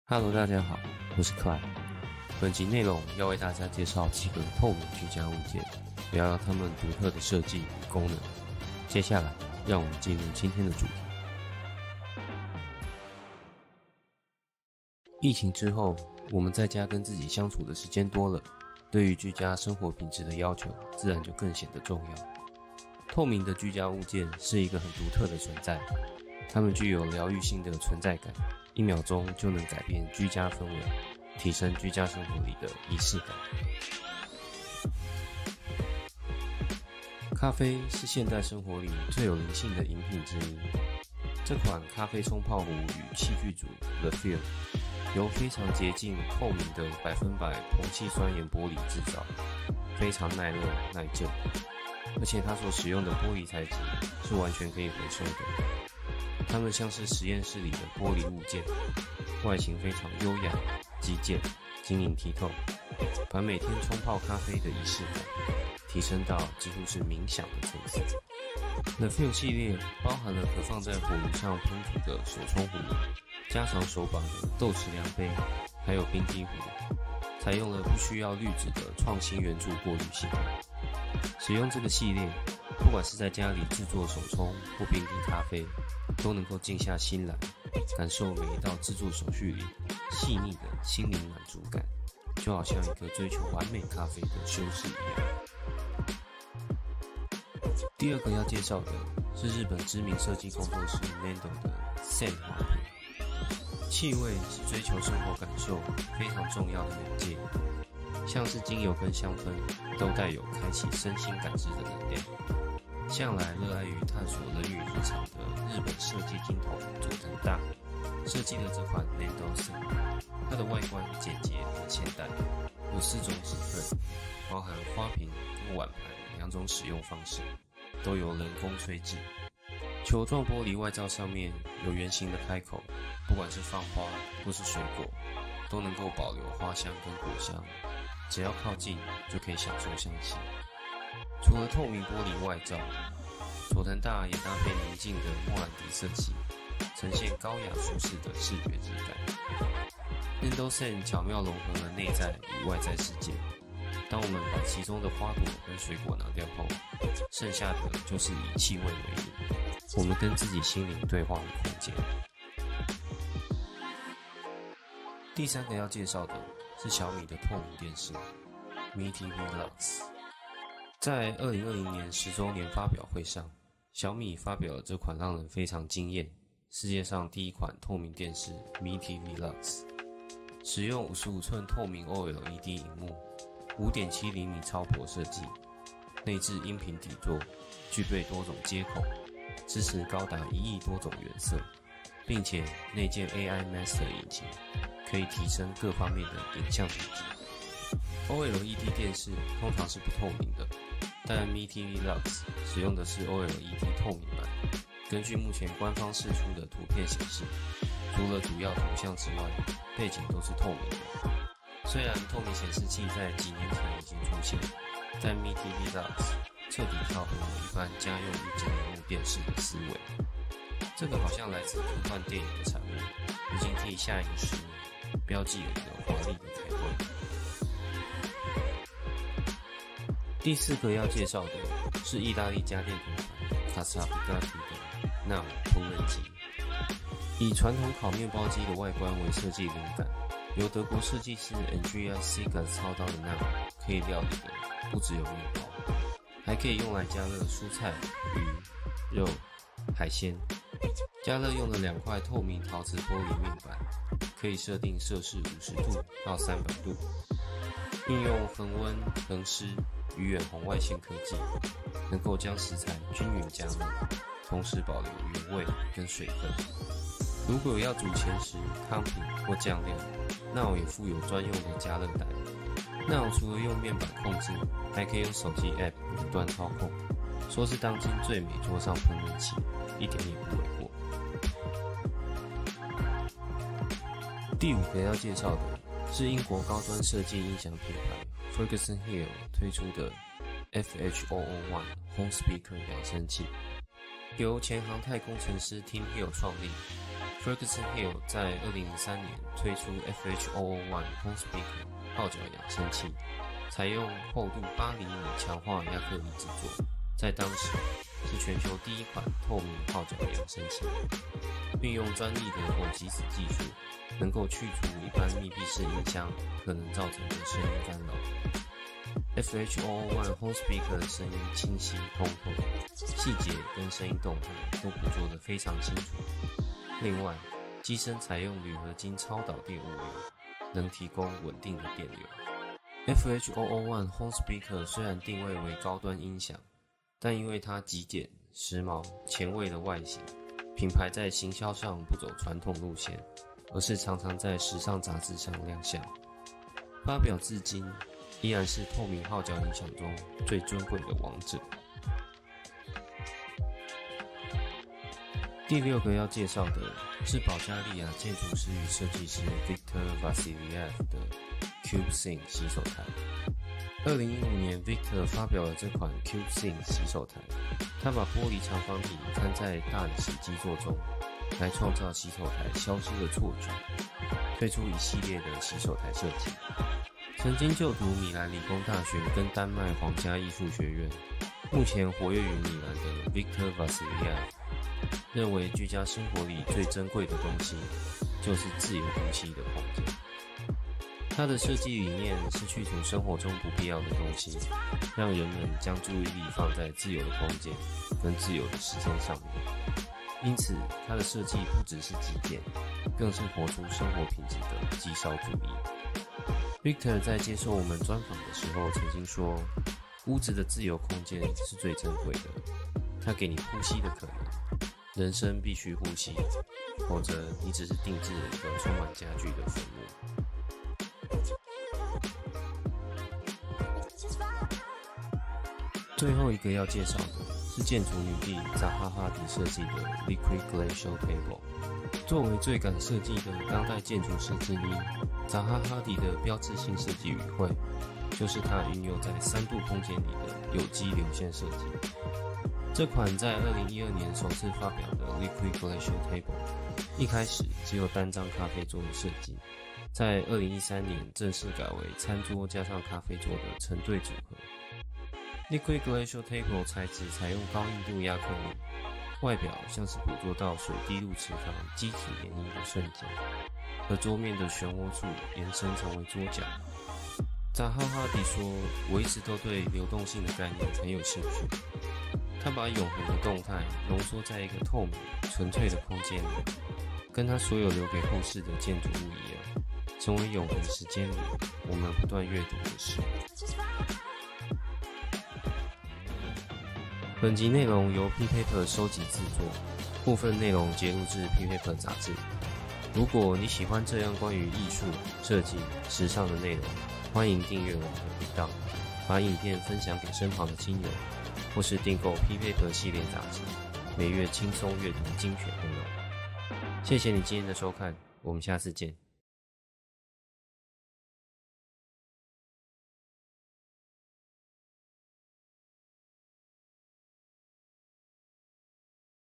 把家變成你喜歡的樣子！7個淨化心靈的絕美透明居家物件｜PPAPER 設計開箱 EP04
在家的時間多了，我們的注意力回到久違的日常起居空間，什麼物件是一日生活不可或缺的存在？什麼擺設在此刻卻突然顯得多餘突兀？現在正是重新整理生活的絕佳時機。本集PPAPER 設計開箱嚴選7件療癒透明居家物件，去思考如何去活得少但是活得美好，把家變成你喜歡的樣子！